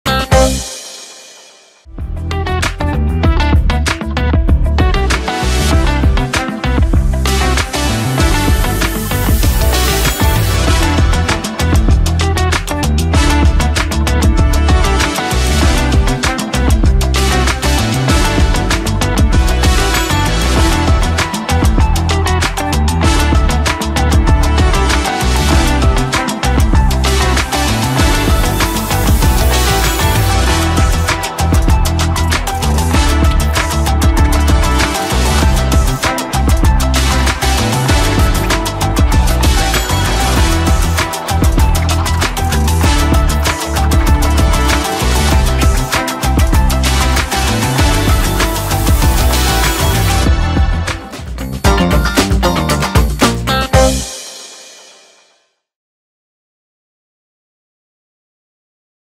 Resumo do primeiro dia do CIC 2023 (23/10)
Resumo do primeiro dia dos Congressos Científicos do Centro Universitário de Adamantina (CIC 2023), em 23 de outubro de 2023